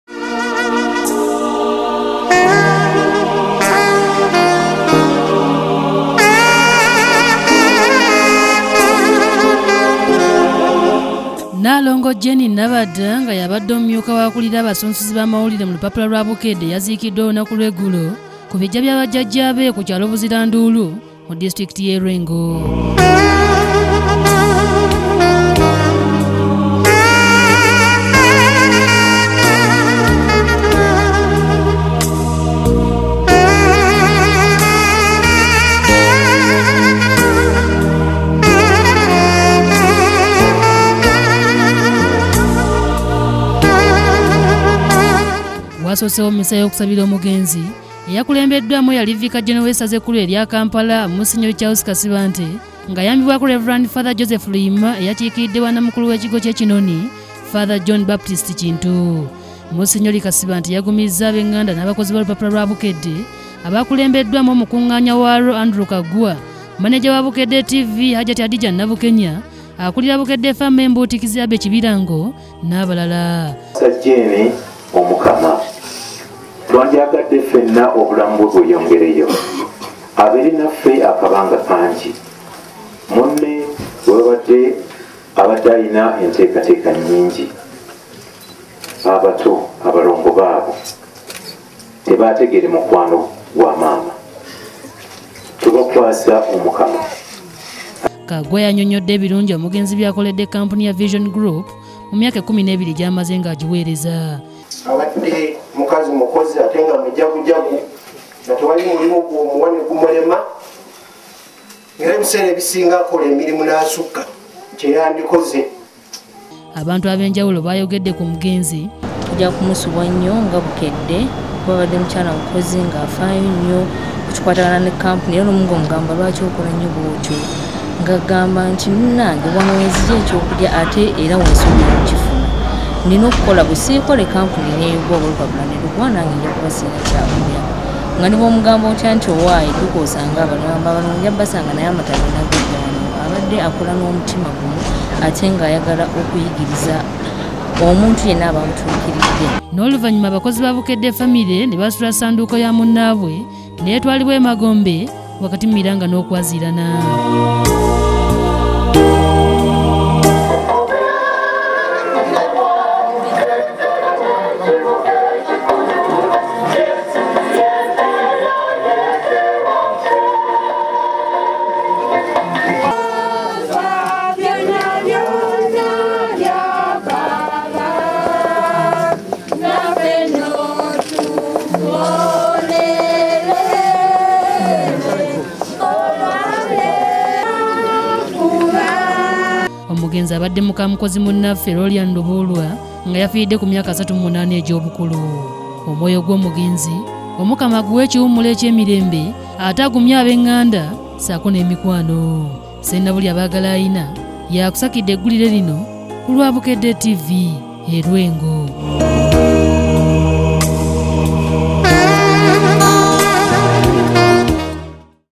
Omukozi wa Bukedde famire aziikiddwa
Alese abalongo ne Ssalongo mu buyinike